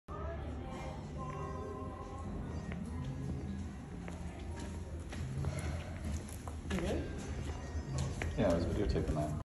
Walking Over Embedded Wine Bottles In Floor at Quinta Da Pecheca, Douro Valley
The Elegant Resort in the Douro Valley has many opulent features including embedded wine bottles in the floor